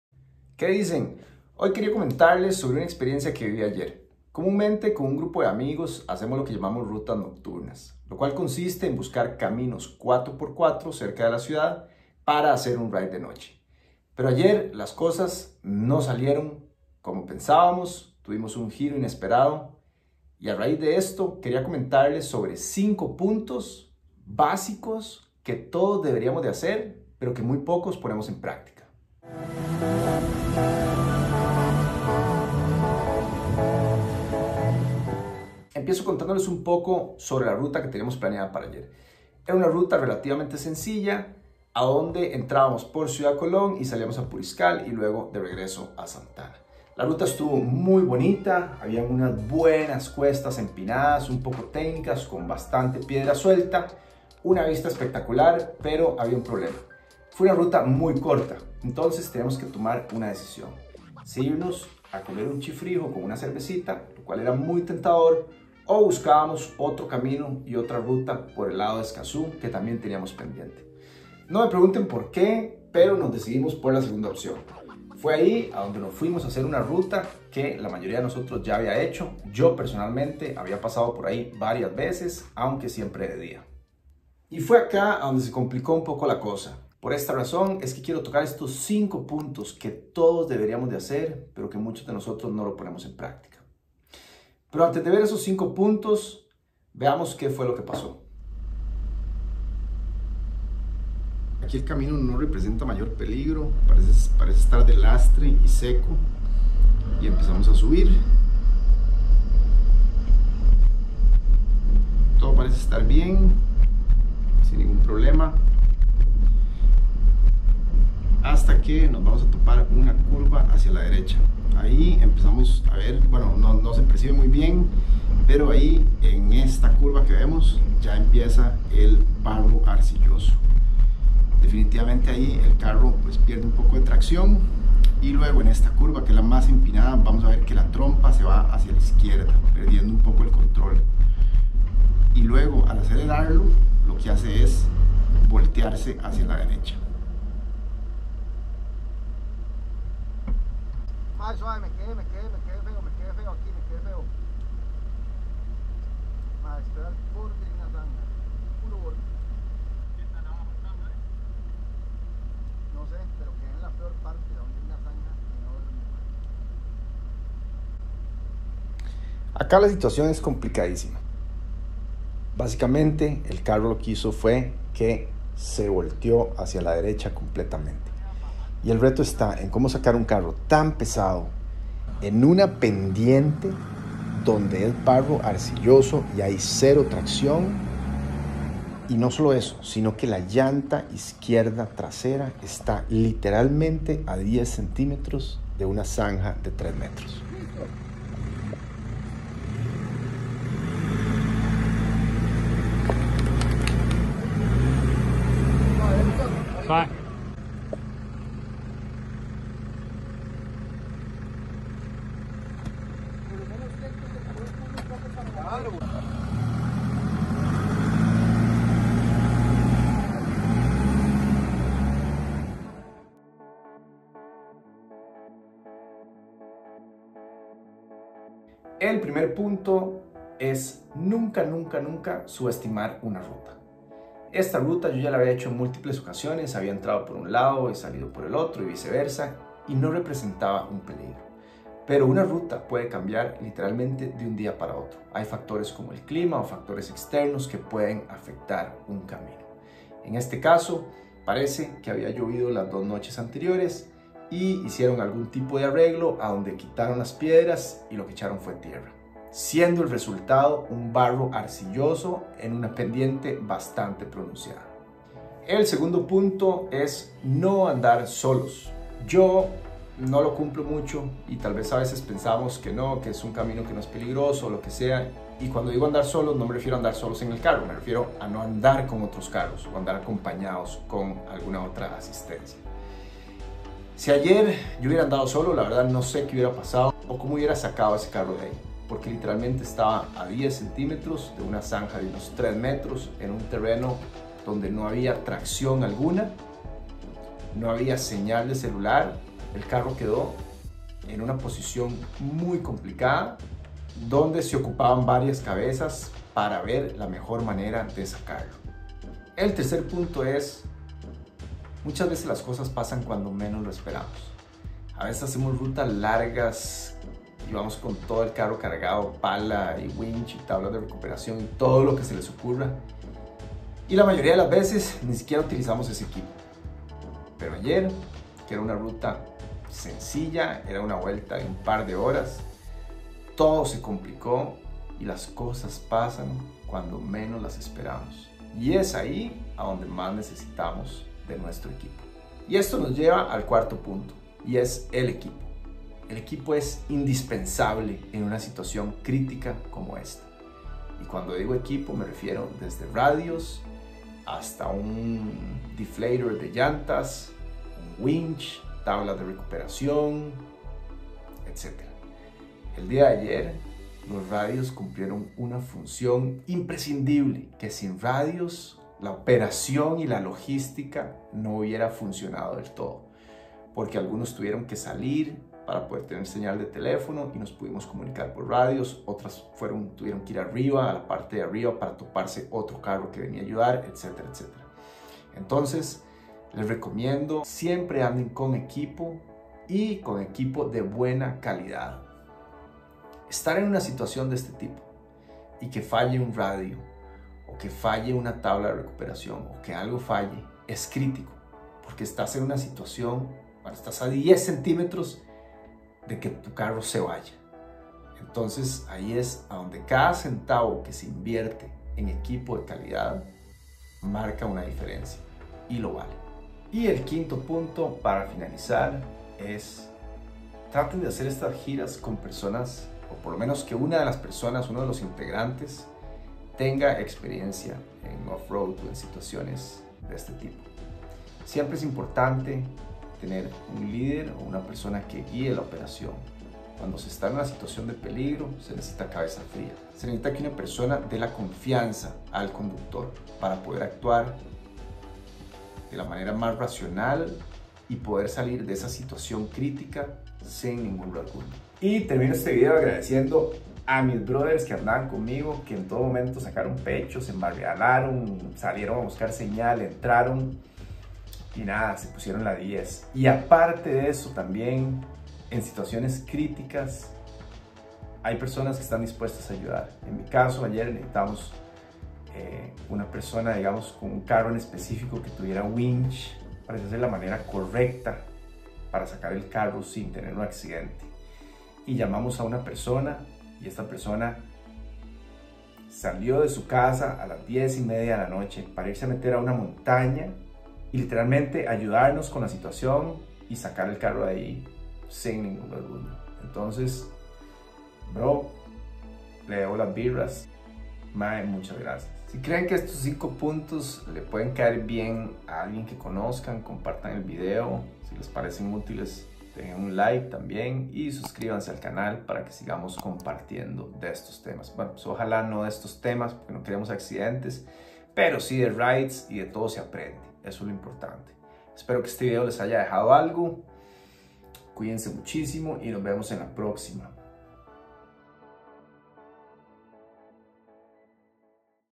4x4 Fail - Cinco Consejos Básicos para evitar imprevistos
No importa si eres un principiante o un experto en rutas 4x4, estos son cinco puntos básicos que siempre debemos de tomar en consideración, pero muchas veces, aunque los sabemos, los ignoramos.

En esta ocasión realizamos una ruta nocturna 4x4, la cual dio un giro inesperado. Les cuento qué sucedió y hablo sobre 5 puntos claves para evitar tener imprevistos cuando andamos en una ruta off-road.